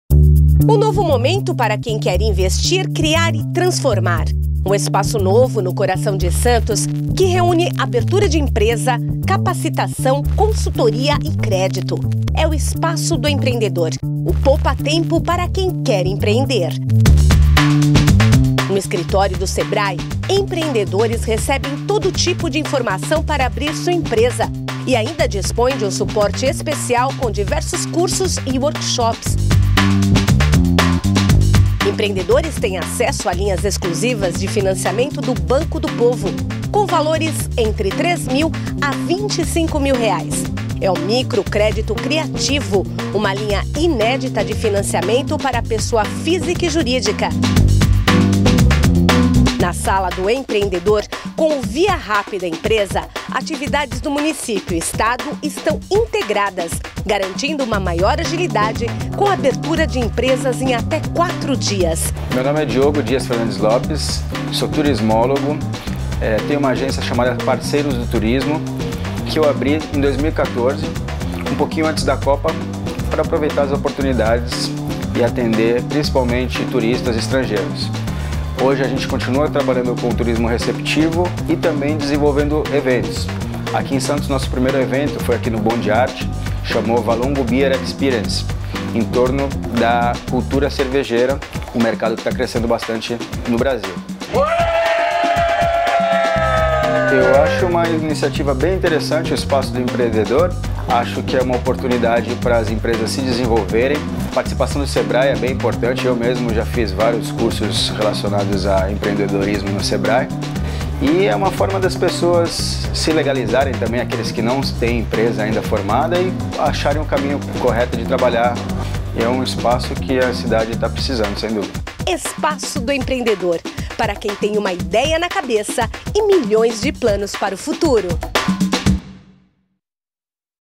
Empreendedor santista tem espaço dedicado para os negócios
Espécie de Poupatempo para os empresários, o local oferece agências do Banco do Povo, do Sebrae, Sala do Empreendedor e do Via Rápido Empresa, que deve entrar em operação em 60 dias, pois é uma plataforma digital que vai operar em parceria com o Governo do Estado e a Junta Comercial do Estado de São Paulo.